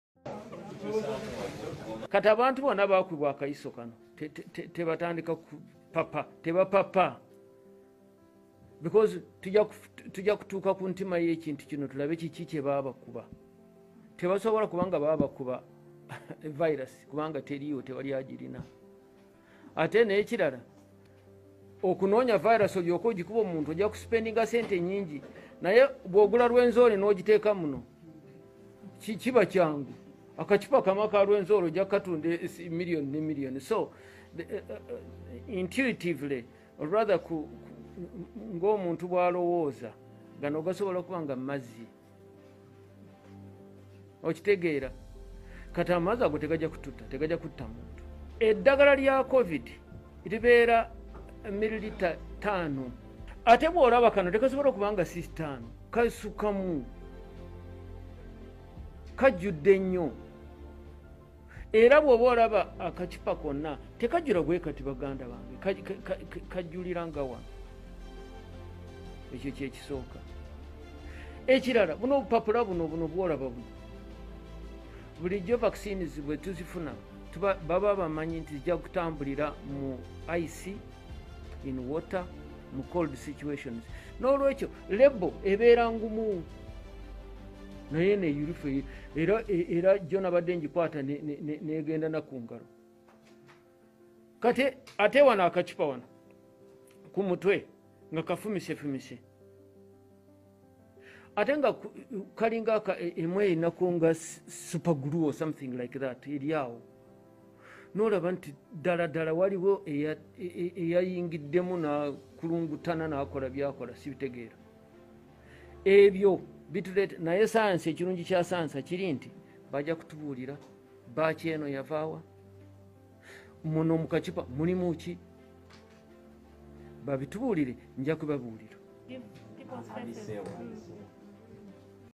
Bitiisa Audio Enkambwe! Omusawo Wa Museveni Abotodde Ebyama Kunfa Ya Bana Uganda
At least 800 people in Uganda have been inoculated with fake COVID-19 vaccines, officials disclosed on Wednesday.

Police, Health Ministry officials, and State House Health Monitoring Unit have arrested two nurses for injecting people with fake shots and issuing fake certificates. The doctor who was leading the operation, however, is on the run.

Addressing a news conference in the capital Kampala, Dr. Wallen Naamara, the head of the health unit, said the network duped many people and companies.

"It is unfortunate that some medical workers are administering fake COVID-19 jabs to unsuspecting people. We did undercover work and arrested two nurses in the act," she said.

According to her, those injected were mostly from companies that paid 200,000 Uganda shillings ($54) for the vaccination of each of their workers.

A representative of the Uganda National Medical Stores, a government-owned organization mandated to obtain, store and distribute medicines, said the items seized from the nurses were not procured from them. The drugs did not have government labeling, as required by law, the representative added.

With 543 new infections and 34 related deaths, the East African country has so far confirmed 79,979 COVID-19 cases and 1,023 fatalities.

It received 964,000 doses of the Oxford-AstraZeneca vaccine in March, and over 800,000 people have since been vaccinated. An additional 175,200 doses arrived in June.

The country's parliament is closed for two weeks until July 11 as 160 people in the complex, including 14 lawmakers, tested positive.